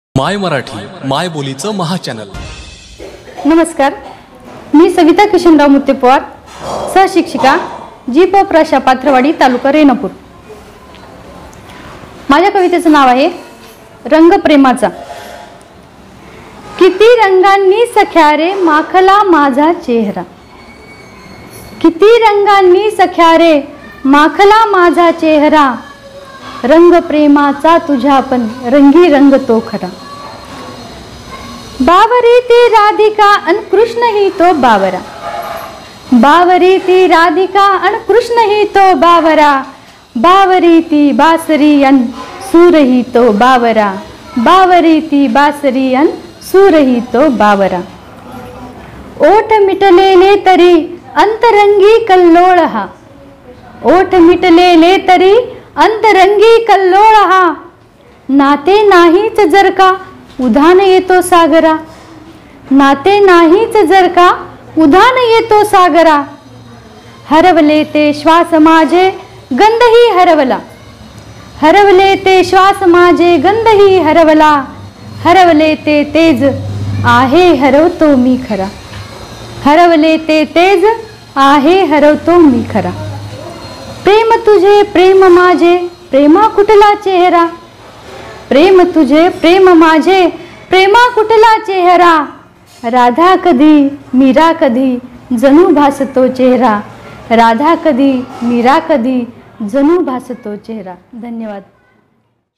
kavita,सविता मुत्तेपवार, savita muttepwar,कविता, गझल, गज़ल,marathi poem on love,gazal,kajal,new poem,
सविता मुत्तेपवार .. लातूरच्या रहिवासी असलेल्या सविता लातूरच्याच रेणापूर तालुक्यातील पाथरवाडी शिक्षिका म्हणून कार्यरत आहेत.त्या विविध आशय-विषयाच्या कवितांचे लेखन करतात.विविध काव्यसंमेलनांतही त्यांचा सहभाग असतो.
त्यांनी सादर केलेली ही हळवी प्रेम कविता खास आपल्यासाठी.

'माय मराठी '.
मायबोलीचं महाचॅनल. !!

mai Marathi...
the biggest channel of the mothertoung.
-rajesaheb kadam